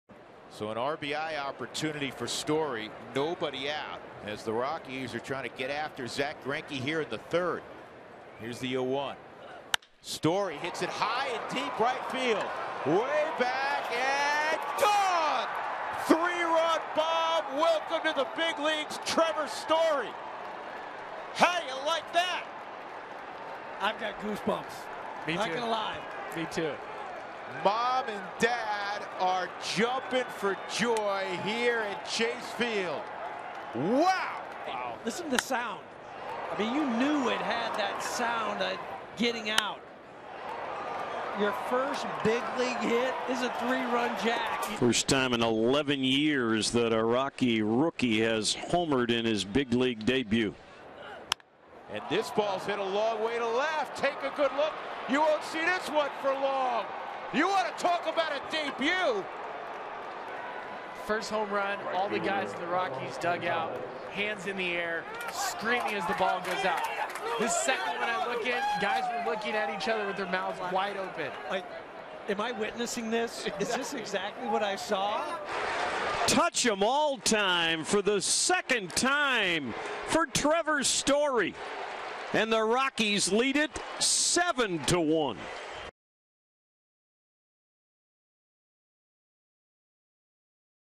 COL@ARI: Story hits two home runs in his MLB debut
4/4/16: Trevor Story belts two homers vs. the Rockies to become the first player with two blasts in their MLB debut on Opening Day